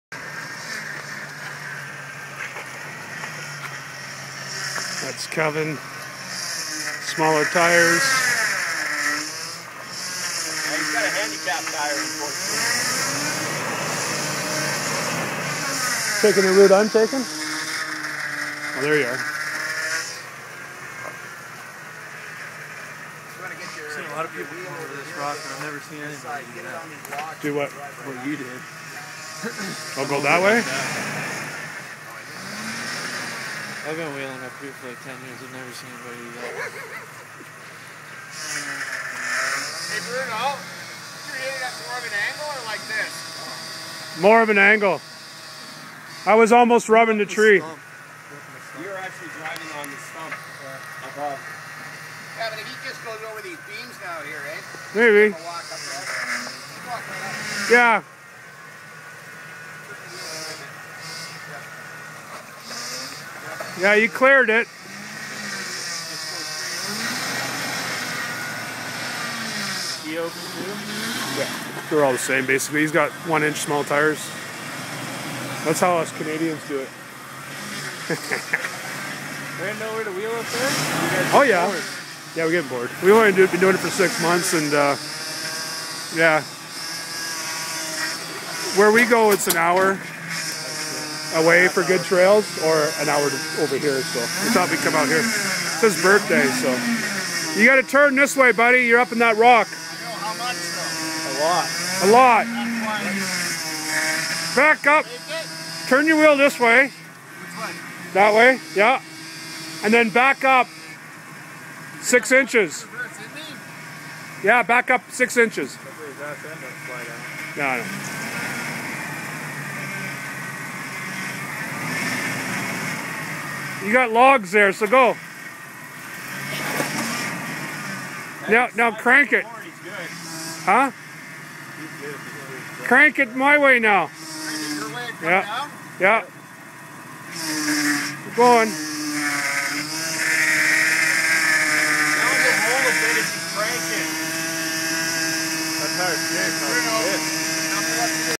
keven going down huge rock
Keven in walker valley going down an 6 foot rock obsticale